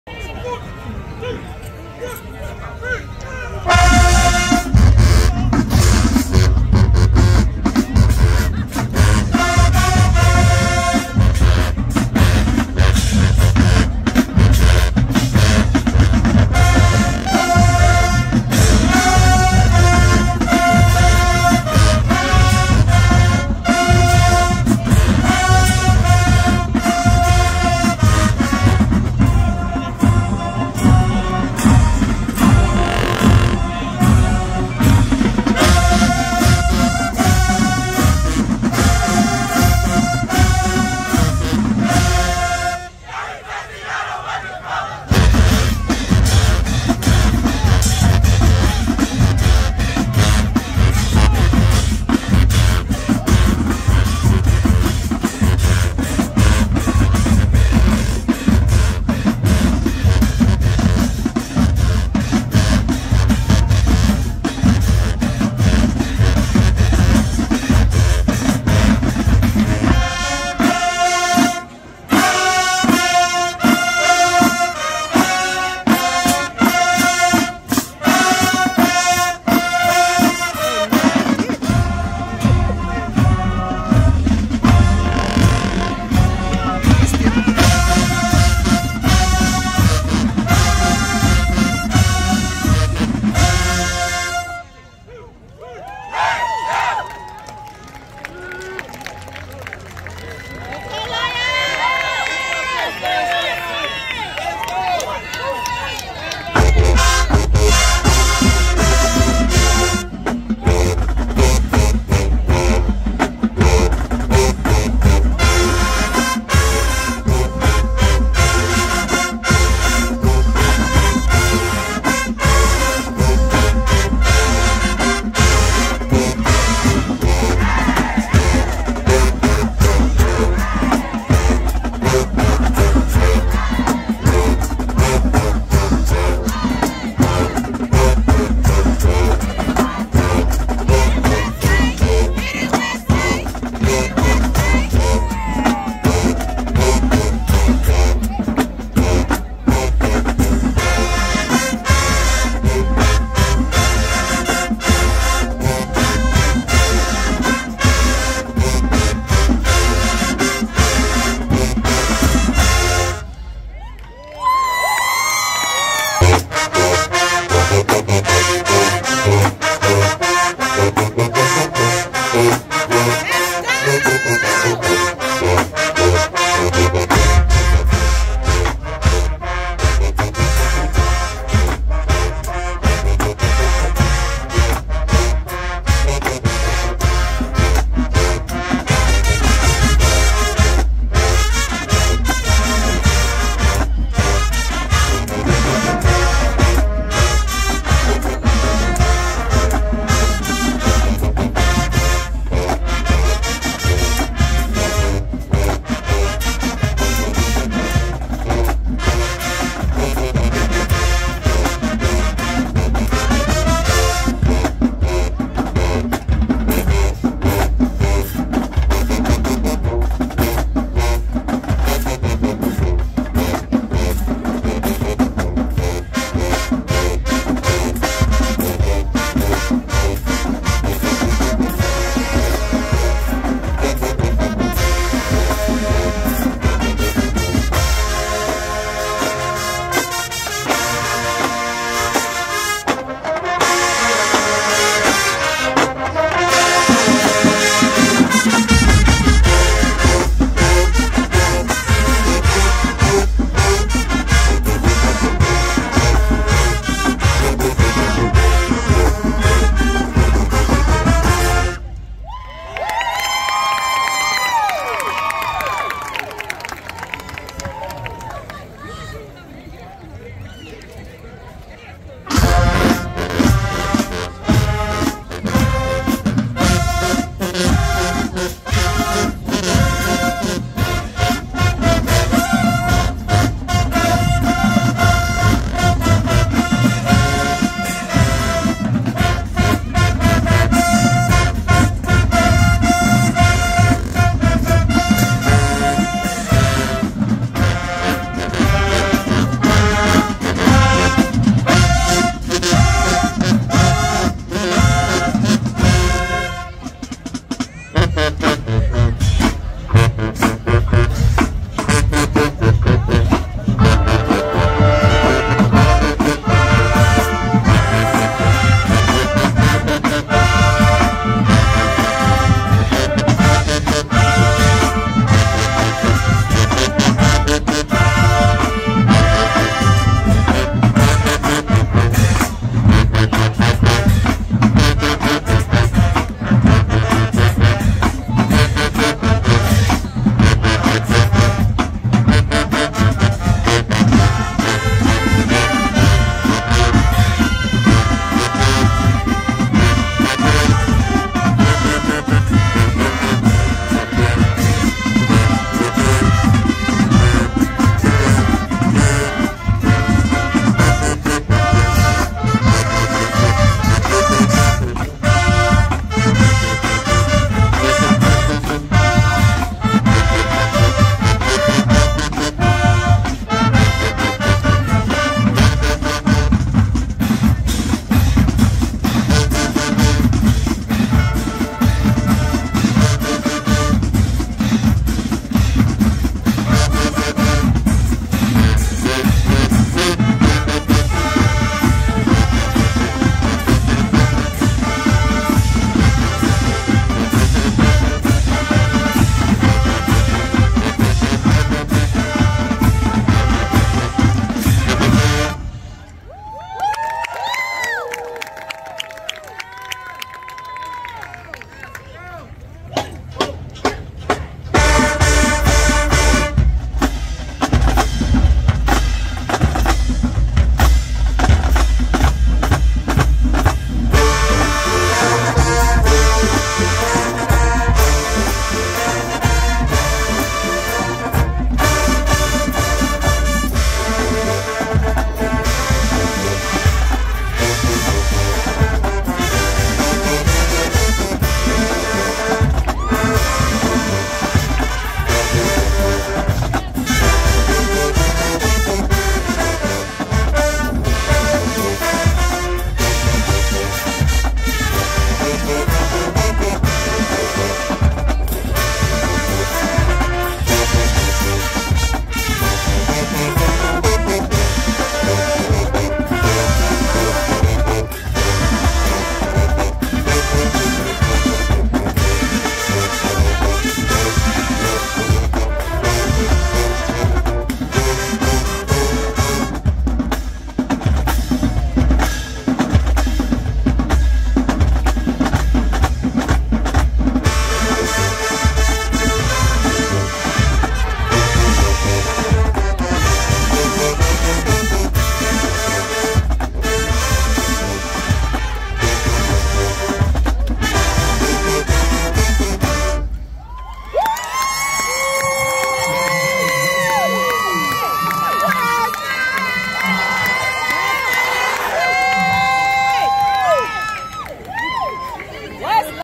Band Battle/ Salem High School vs. Westlake High School/ The Grand Royale Marching Invitational 2024
Salem High School Marching Band vs. Westlake High School Marching Band/The Grand Royale Marching Invitational/ The D DUB/ November 9, 2024/ Austell,  GA